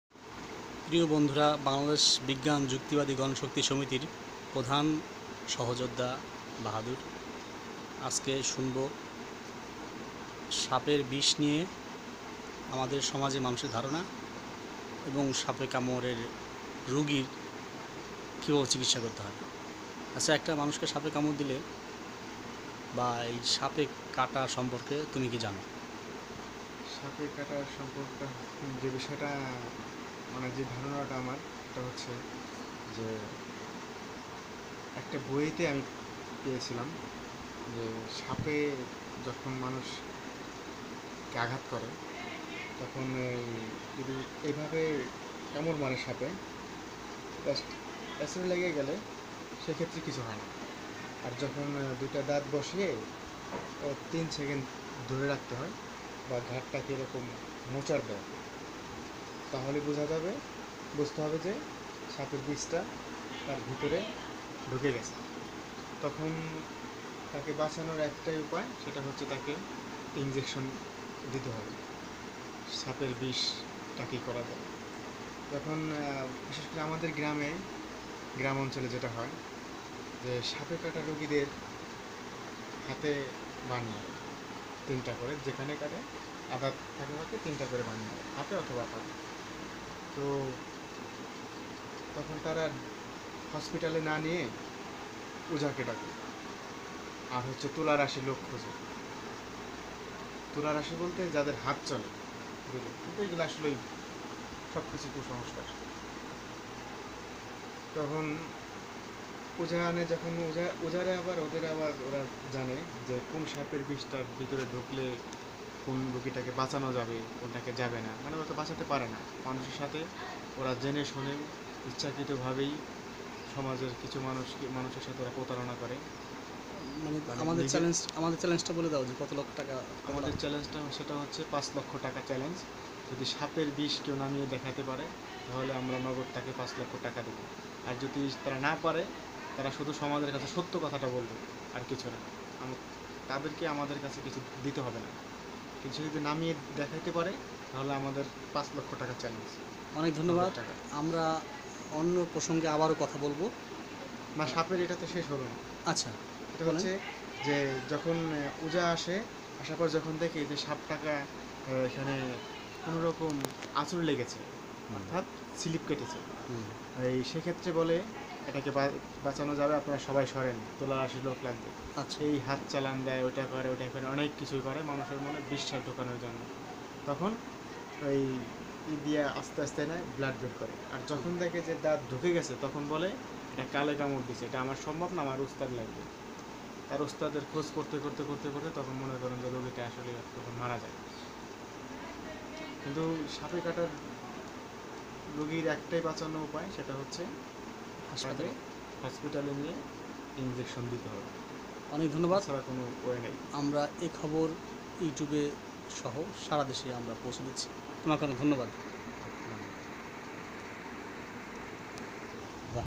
৫ লক্ষ টাকার চ্যালেঞ্জ।বাহাদুর যু্ক্তিবাদী
সব্বাই জানিয়ে দেয়ার জন্য অনুরোধ করছি।সাপে কাটা রোগীকে বাচানোর উদ্দেশ্য আমাদের, বাংলাদেশ বিজ্ঞান যু্ক্তিবাদী গণশক্তি সমিতি এর।